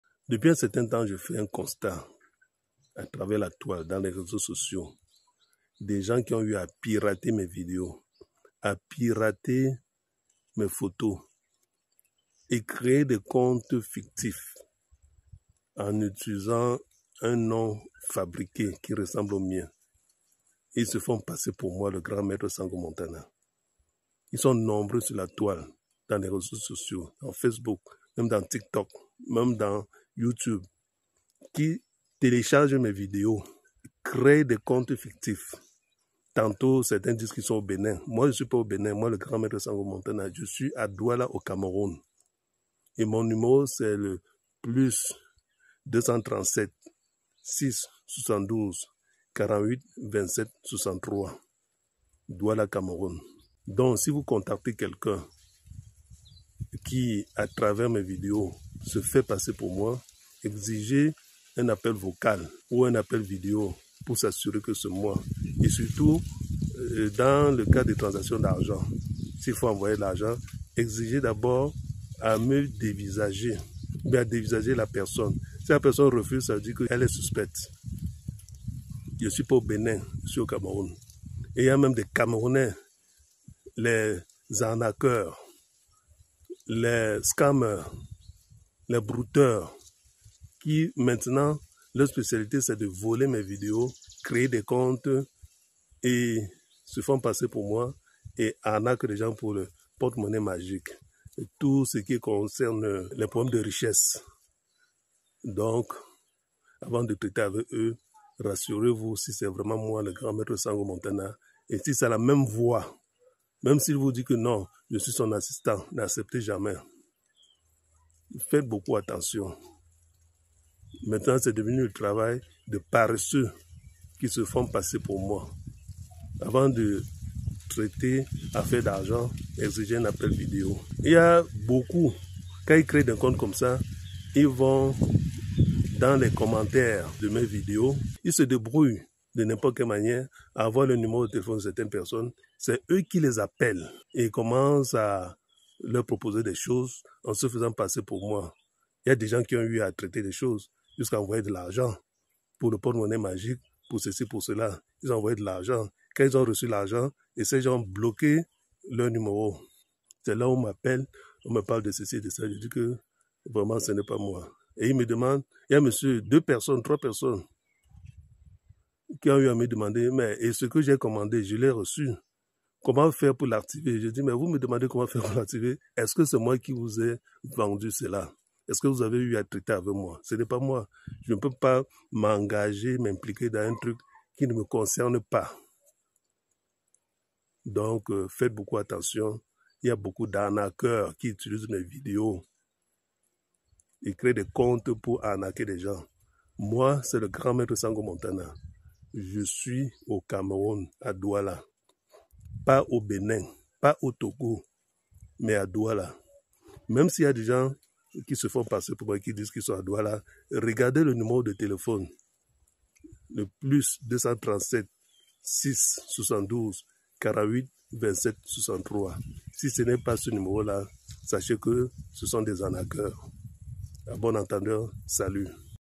DES BROUTEURS PIRATENT MES VIDÉOS POUR ARNAQUER
Cette vidéo est pour demander à mes abonnés d'être vigilants désormais, car des brouteurs, des SCAMMERS, des arnaqueurs piratent mes vidéos, mes photos et créent des faux comptes avec des noms fictifs et escroquent beaucoup de personnes. ils sont basés au Bénin principalement, au Togo et au Cameroun. Je suis à Douala au Cameroun et non au Bénin.